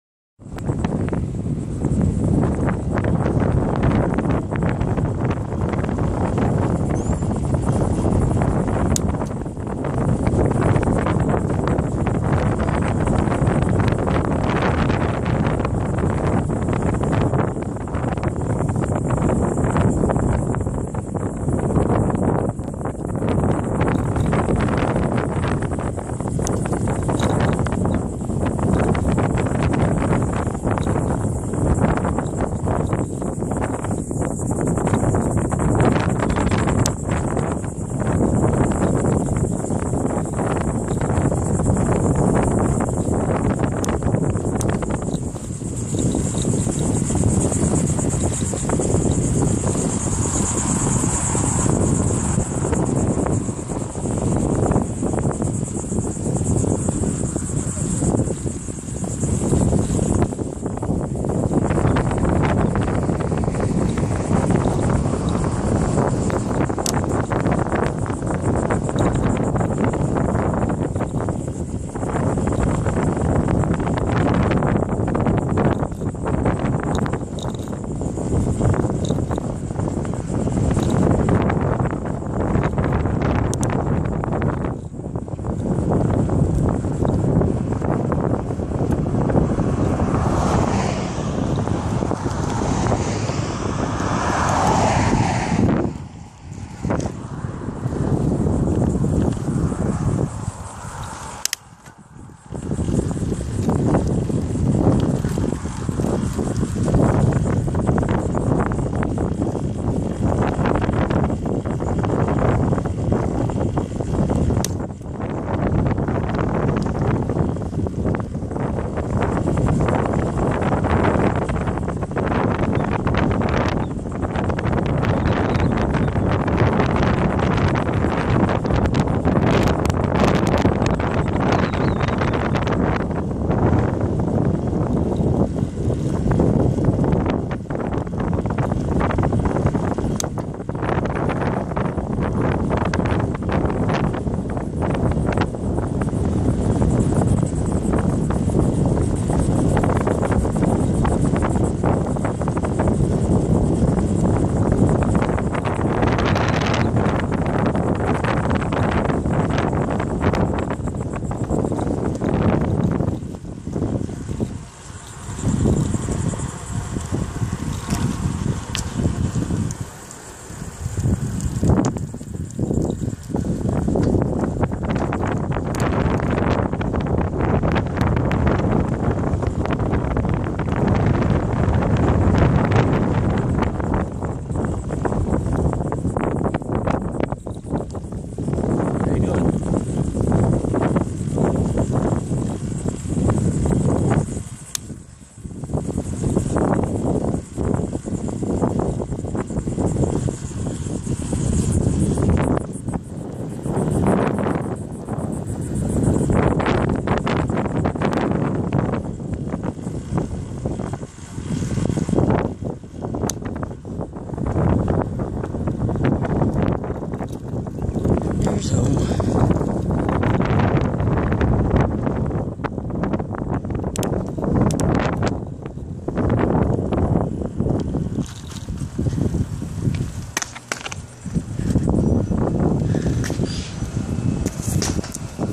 A short video from the end of one of one my lesure rides on the OBX.
Apologies for the shaky video.  I shot this while holding my phone in one hand and steering with the other.  The bike was a Bachetta Corsa recumbent.  The location is South Beach - Salvo, North Carolina.  The camera was a Samsung Galaxy S4 smartphone.